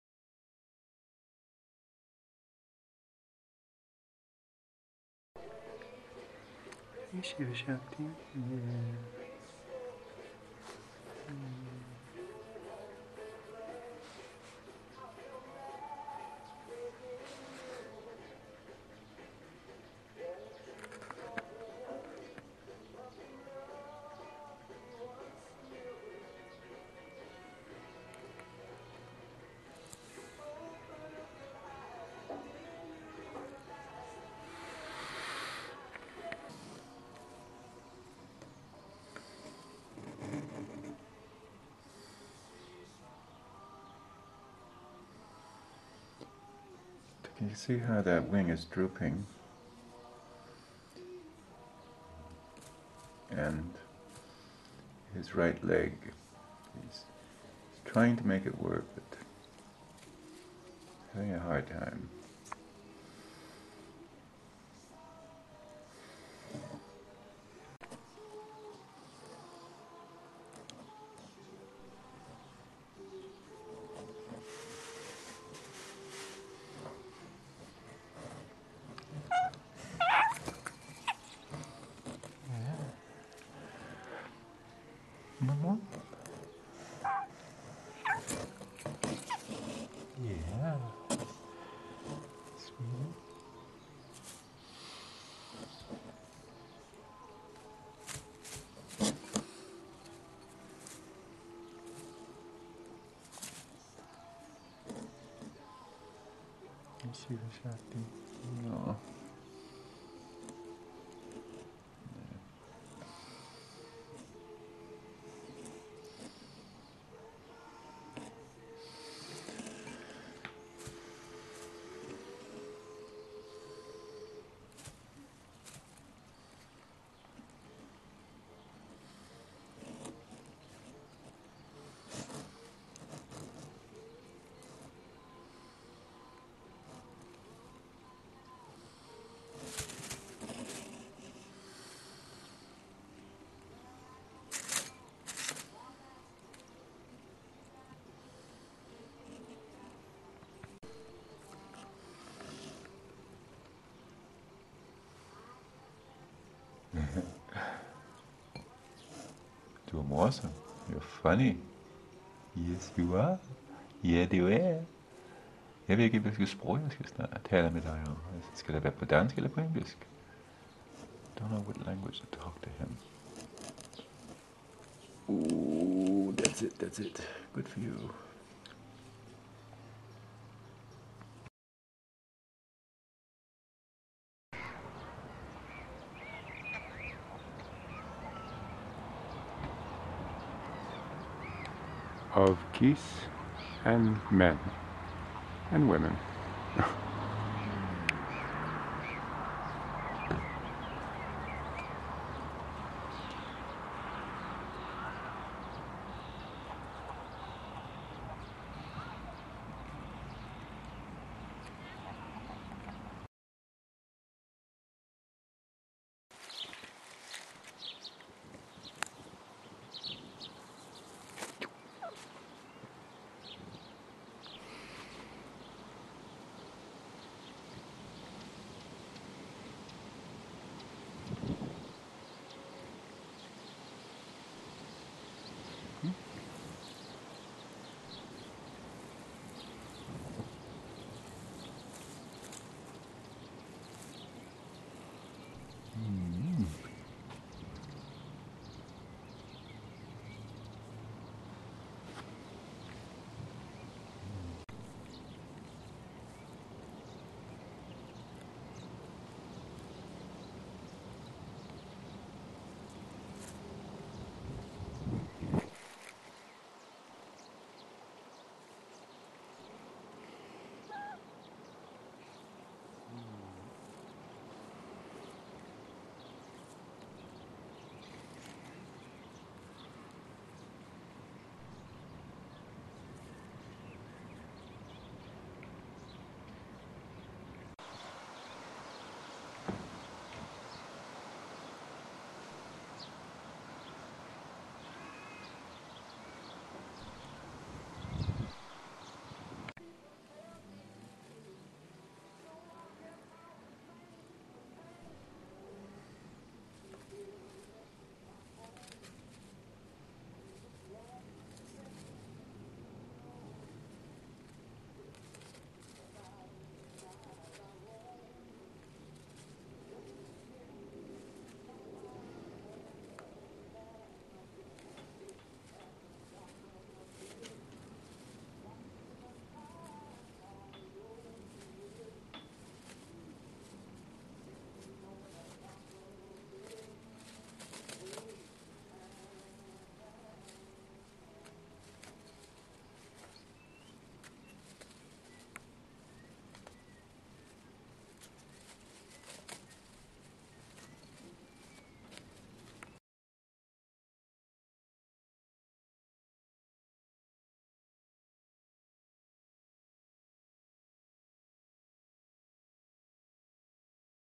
Sweetie/Shivie Danish Hooded Crow (Gråkrage) in Rehab
May 26-28 scenes from Shivie's remarkable recovery, with short 20 second intermission: "Of Geese and Men (women)," from Utterslev Mose wetlands of Copenhagen